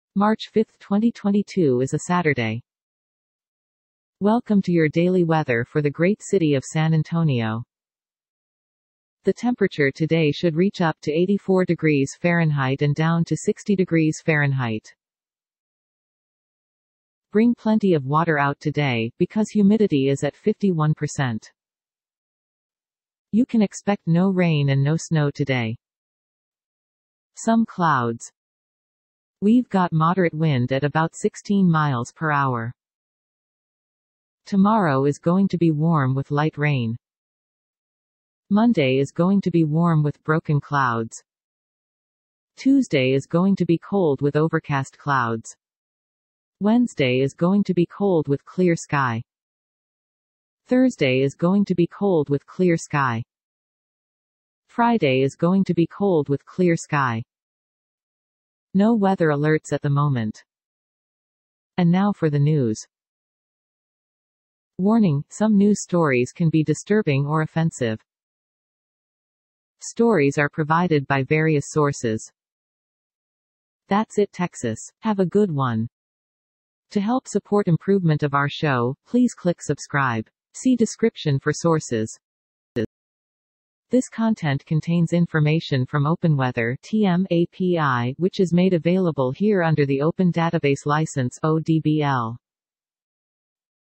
News and Weather Forecast for Saturday March 05, 2022 in San Antonio, TX
The daily news+weather for Texas.Weather Forecast for San Antonio on Friday March 2022. This is an autogenerated weather and news report.